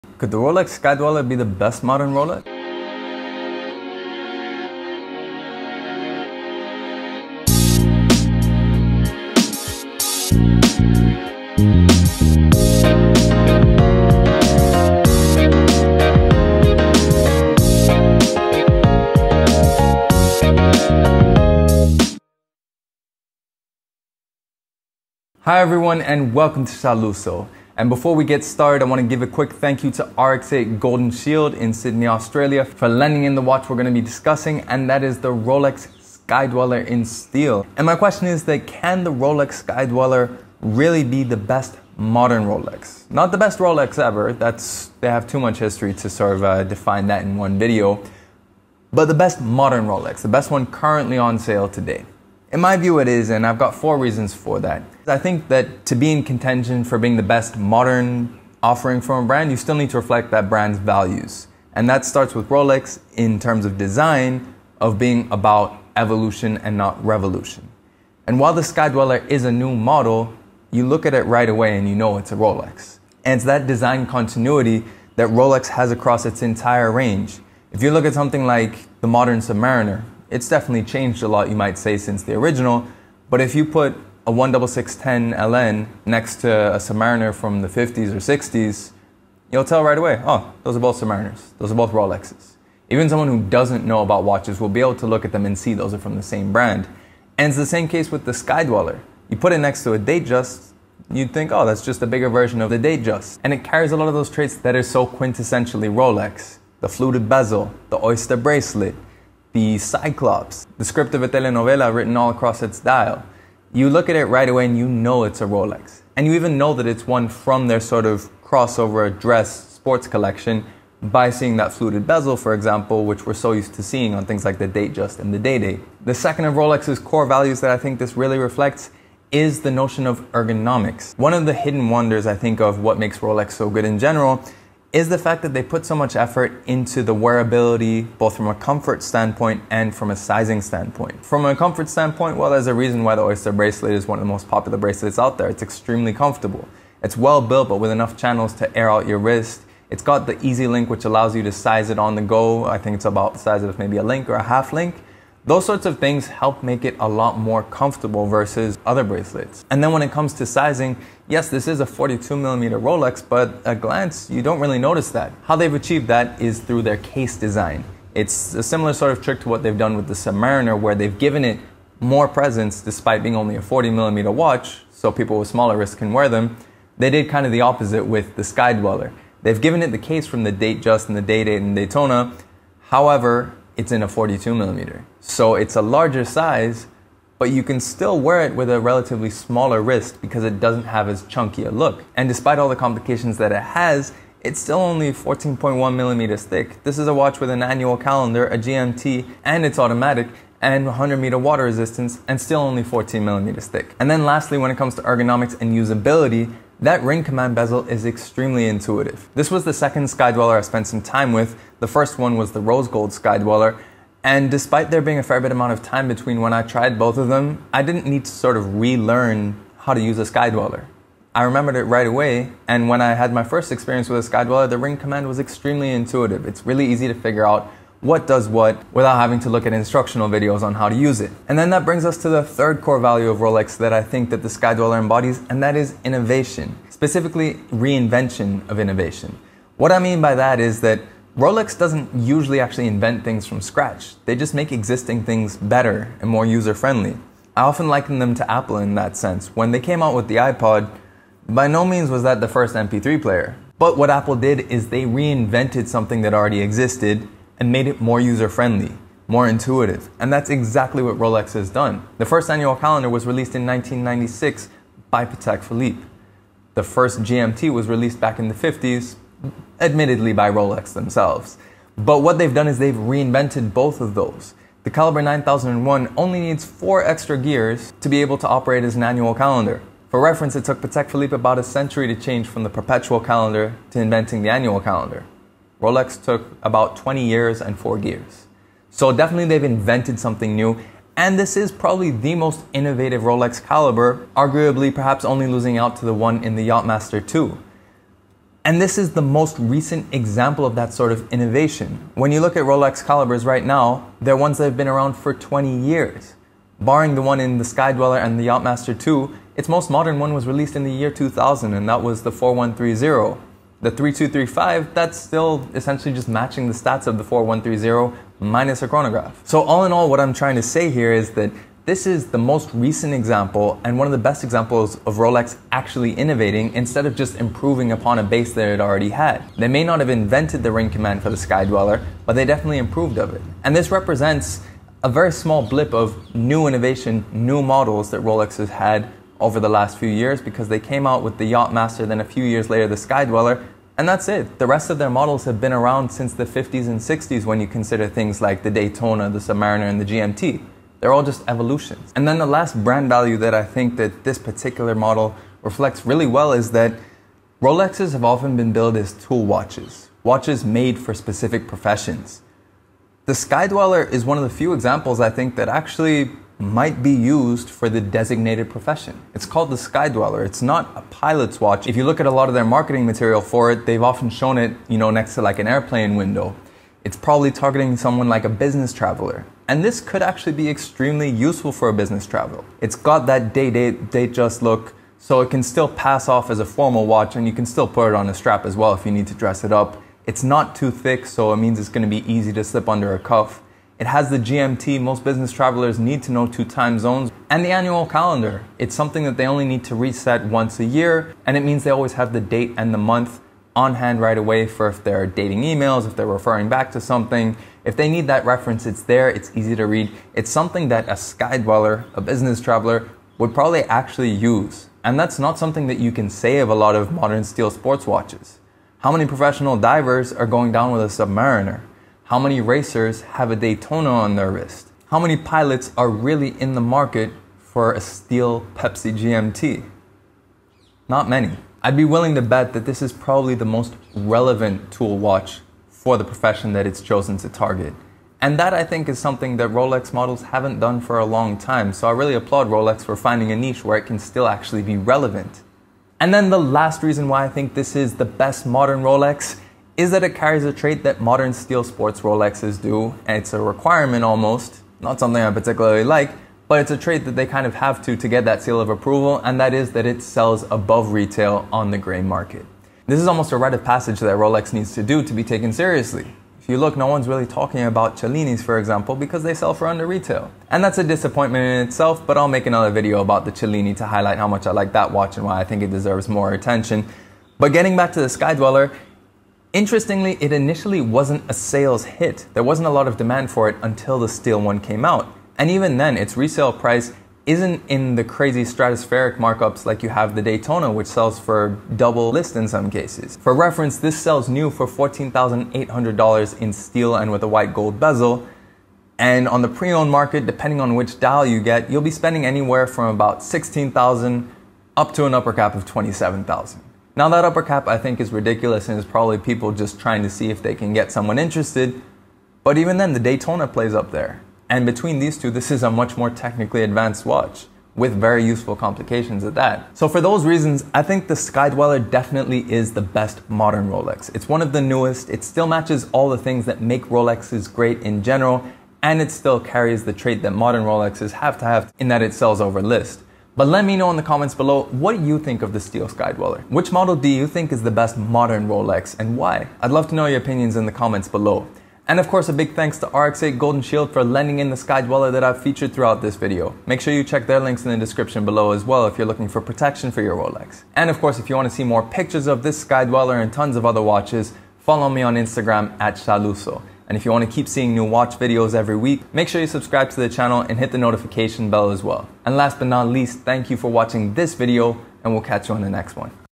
Is the Rolex Sky Dweller the best Modern Rolex? | Steel Rolex Skydweller 326934
The Rolex Skydweller may not be the first model that comes to mind - but could it possibly be the best modern Rolex on sale today? In my opinion, it is. To be clear I'm not saying its the best Rolex ever, but the best example of Rolex as a brand from its current model lineup.

The Skydweller, specifically the steel Sky Dweller (Ref: 326934) is a stunning example of 4 key traits of Rolex in general, as well as one very specific one to modern Rolex:
0:00 Intro
1:00 Design continuity, evolution & brand Identity
2:05 Ergonomics, sizing & user-friendliness
3:51 Innovation of the Calibre 9001 and the Ring Comand bezel
5:52 A modern example of a genuine tool watch that's used by the professionals its made for
7:26 The steel Rolex "scarcity" and resulting premiums affecting the price of the steel Skydweller

Rolex is the most recognizable brand in the world of watches, and part of that is down to its commitment to a singular design language - the only collection that has endured dramatic changes over the years is the Cellini, but even its newest model - the Skydweller - is instantly recognizable as a member of the Day-Date & Datejust family.

Meanwhile, on the inside, the calibre 9001 of the Skydweller features one of the most recent examples of true innovation (versus technical evolution) in the form of the ring command bezel, an annual calendar, and GMT function.

Another key trait of modern steel Rolex is that it sells for over its list price on the grey market, and is very difficult to find in retail stores. The Rolex Sky Dweller blue dial is a perfect example of this, however the black dial steel Sky Dweller and white dial variants don't have the same crazy premiums, selling much closer to their list price.

What do you think of the Steel Skydweller? If you don't think the Rolex Skydweller is the best modern Rolex, which model would you say is? What do you think matters the most in deciding on the best Rolex?

Thanks to RX8 Golden shield for providing the Steel Rolex Skydweller 326934 featured in this video. If you're interested in learning more about RX-8 Golden Shield visit:

Do you have a nice watch for me to review?